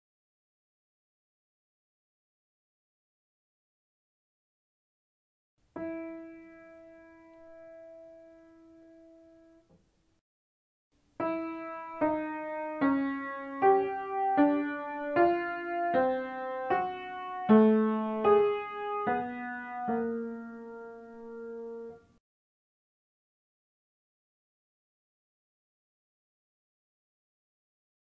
Tone rows (12 tones), lesson 5_Zwölftonreihen, Aufgabe 5 | Ear training | Gehörbildung
Tone rows (12 tones)

In this task type you have to figure a tone row of 12 tones. Every note of the chromatic scale is used once.

Zwölftonreihen

In diesem Aufgabentyp sind Zwölftonreihen zu hören. In einer Zwölftonreihe wird jeder Ton der chromatischen Skala exakt einmal benutzt.